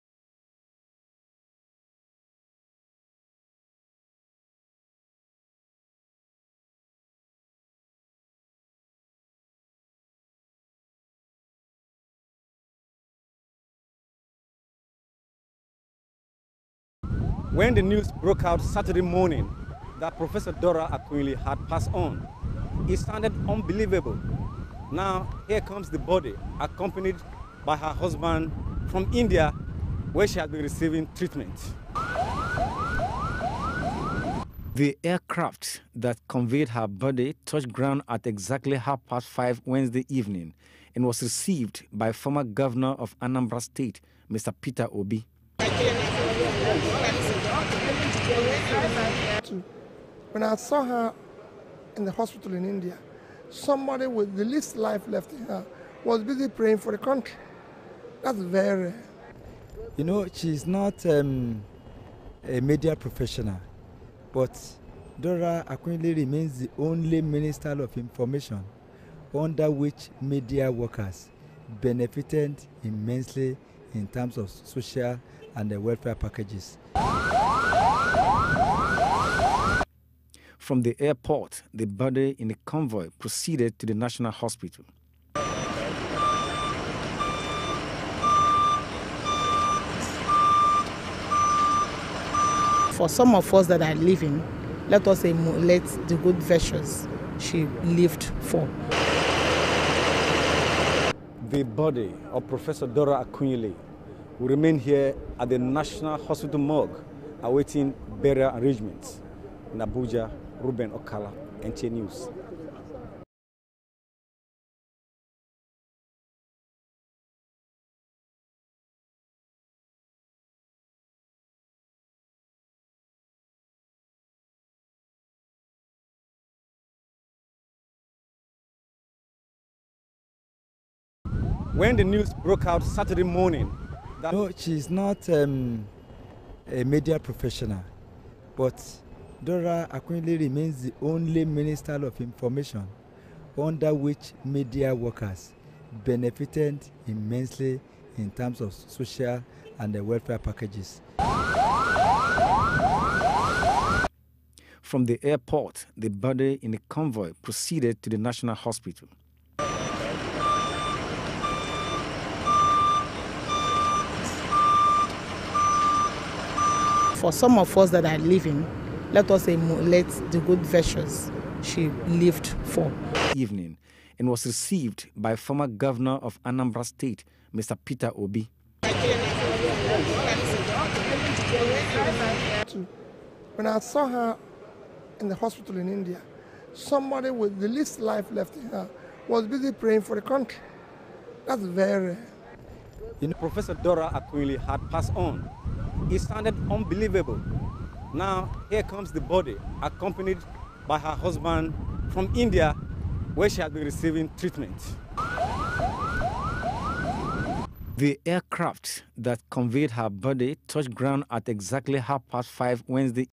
Late Prof. Dora Body Arrives
By Reuben Okala The body of late former Information and Communication Minister Prof. Dora Akunyili this evening arrived Abuja.